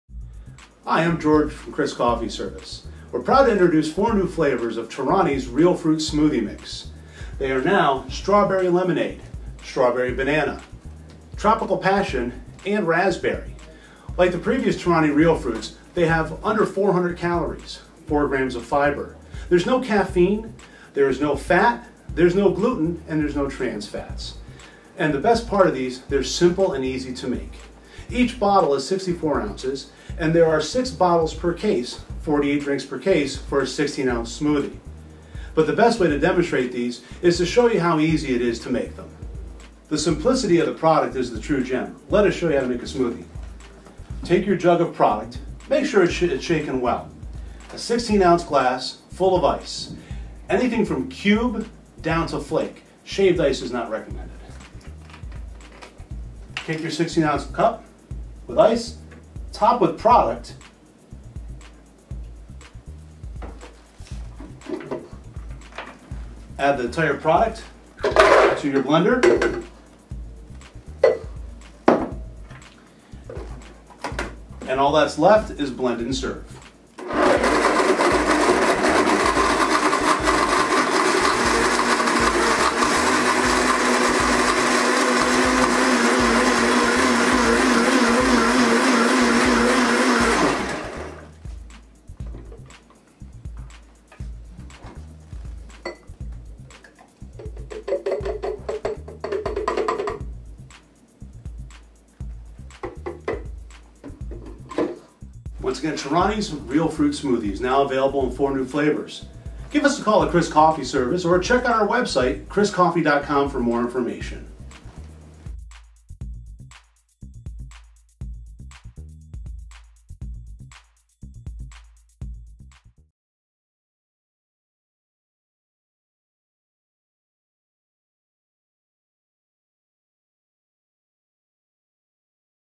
New Torani Real Fruit Smoothie Flavors
George Shoemaker of Chris' Coffee Service goes over the new flavors of Torani's Real Fruit Smoothies and shows how easy it is to make one.